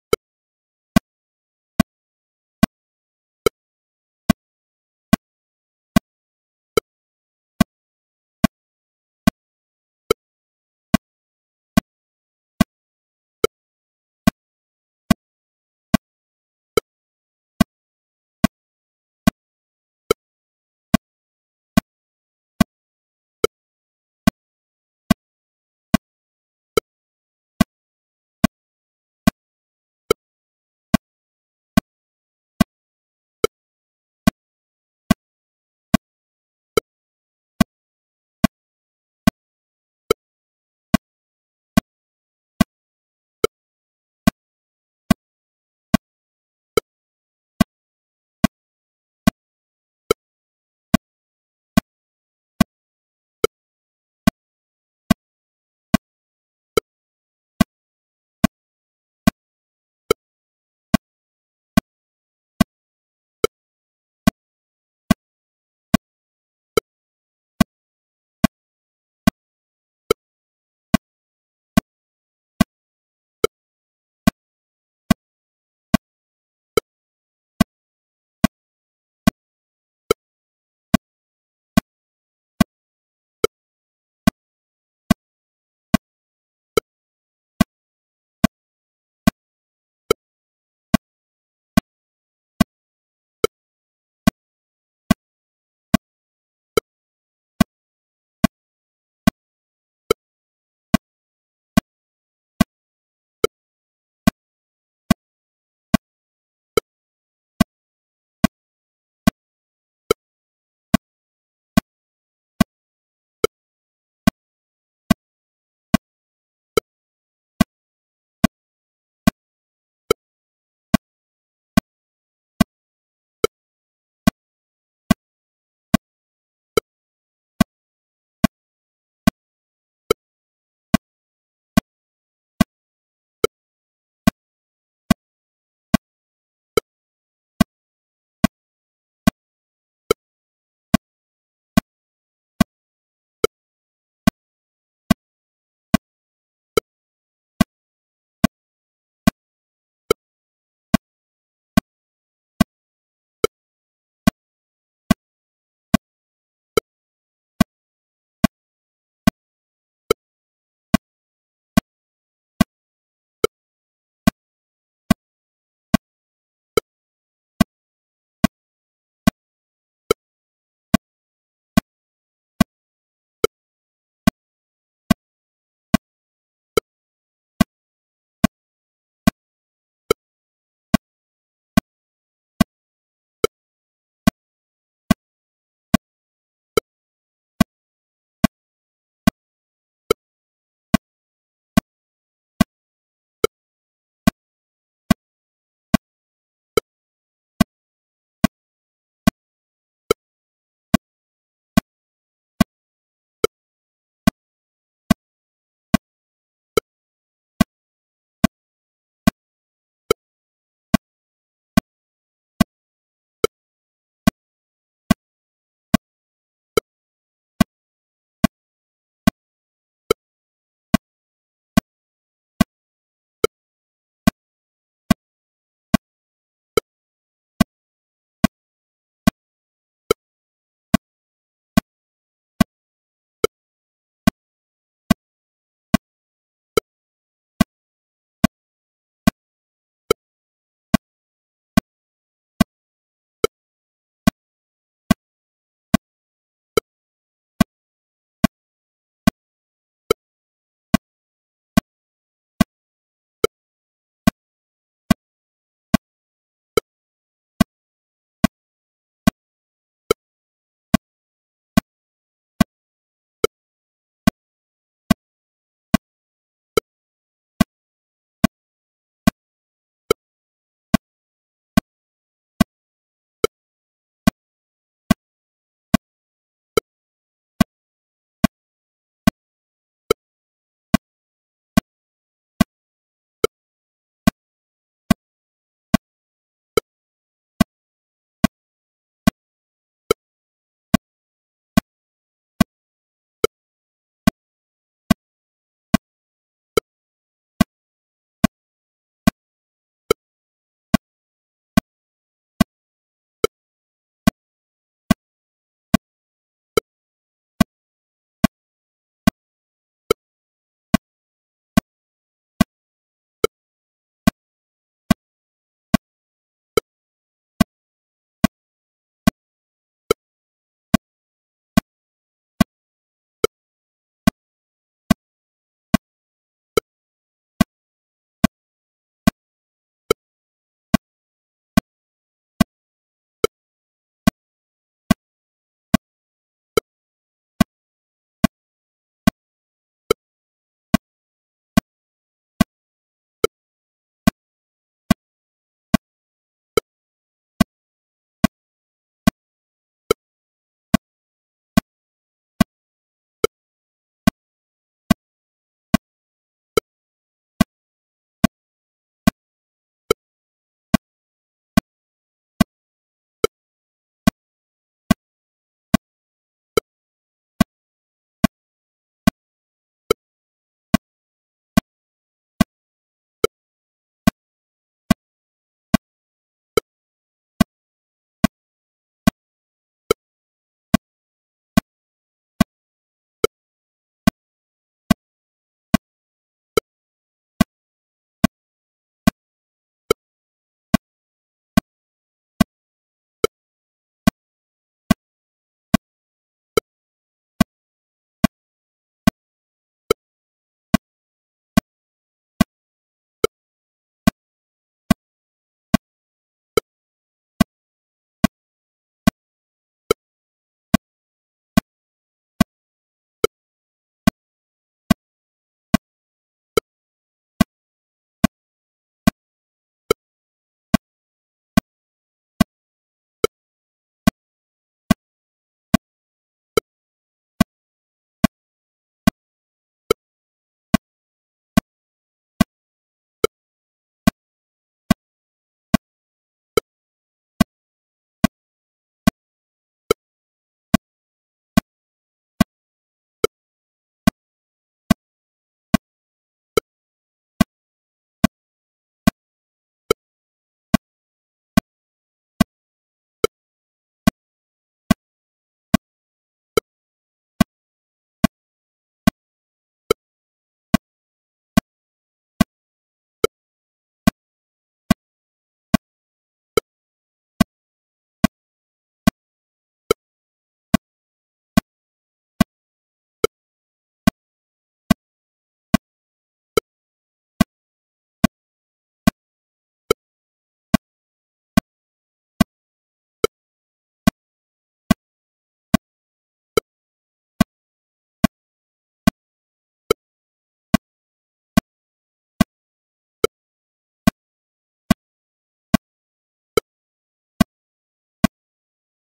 Metronome 72 bpm 4/4 with count
Metronome click track to practice. Also added a count to know in what beat you are. For drummers, guitar and bass players, and anyone who need it. Let me know what other tool you find usefull

Metronomo. Canal de click para practicar. Tambien una cuenta visual para saber en que beat se encuentran. Para bateristas, guitarristas, bajistas y cualquiera que lo necesite. Haganme saber que otras herramientas les resultarian utiles